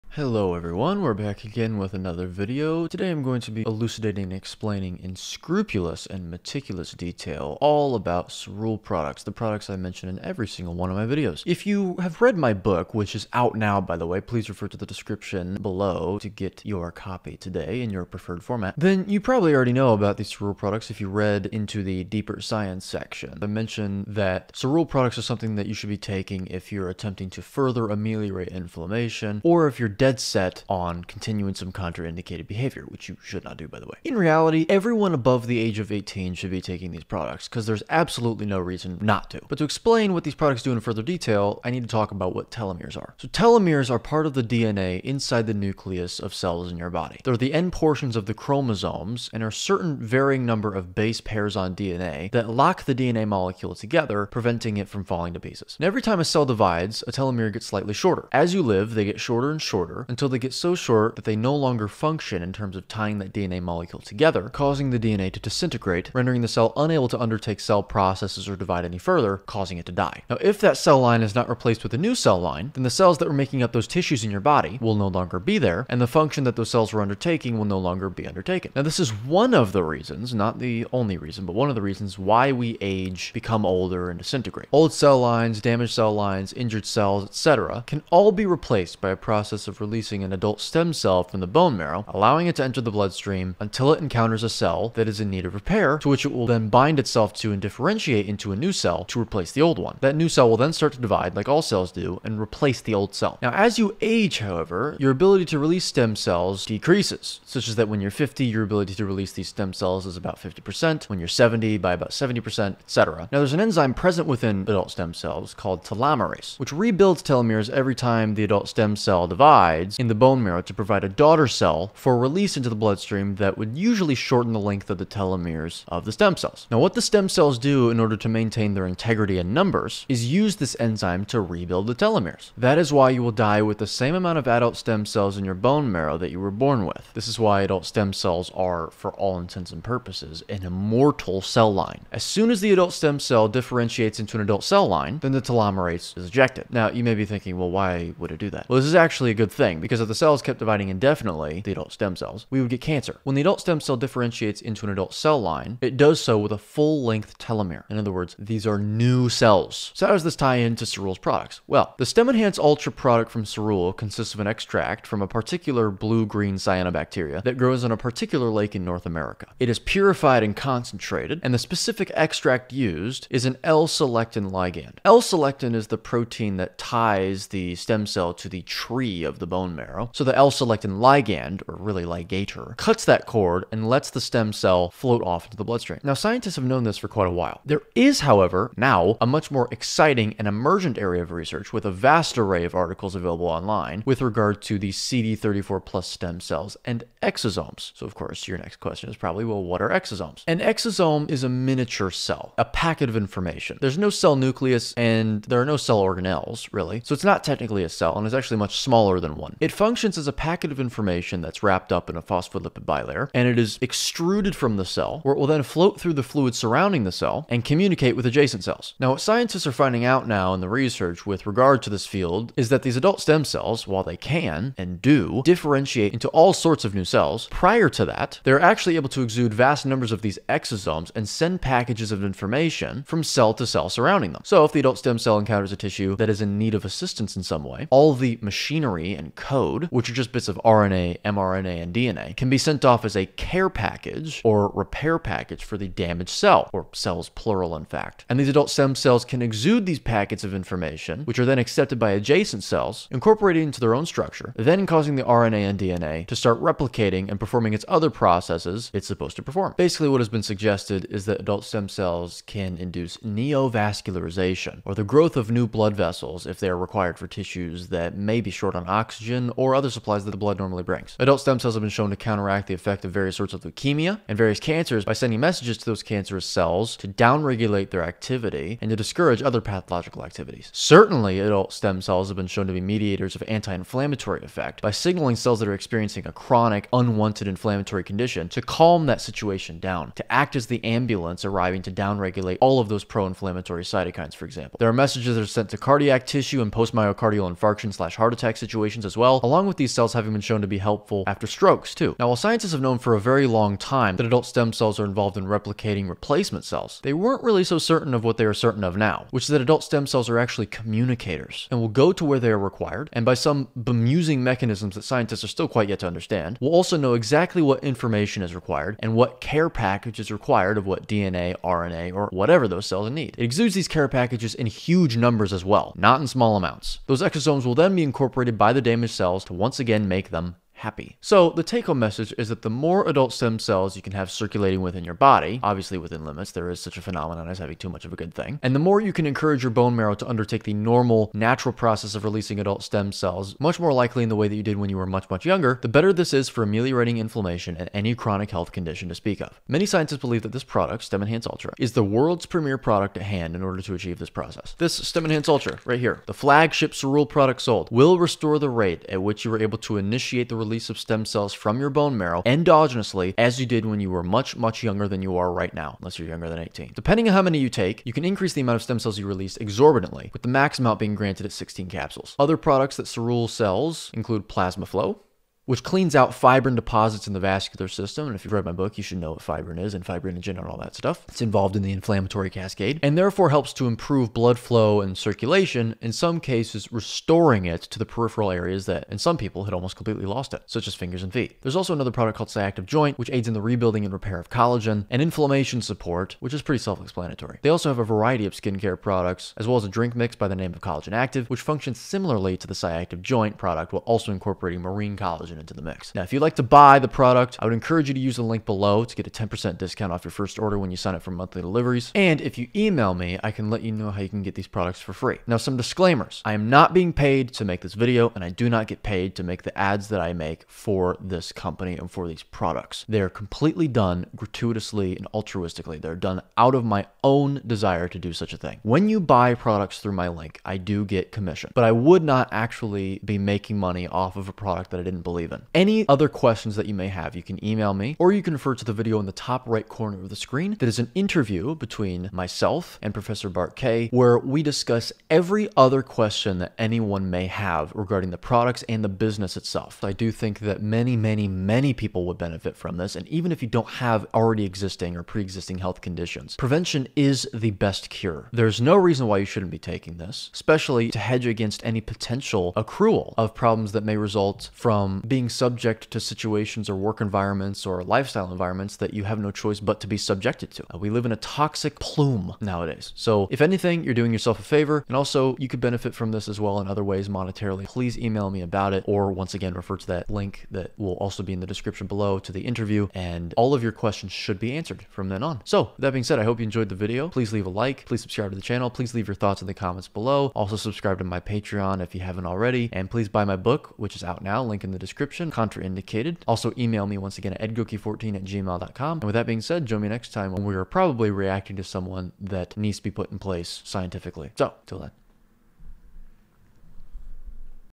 CERULE Products (Updated)
This video is a complete elucidation of the Cerule products I mention in each of my videos on YouTube. Hope you learn something new!

— 1 week early
— Ad-free
— Uncensored
— With Un-Blurred On-Screen References

Help me get the message out there!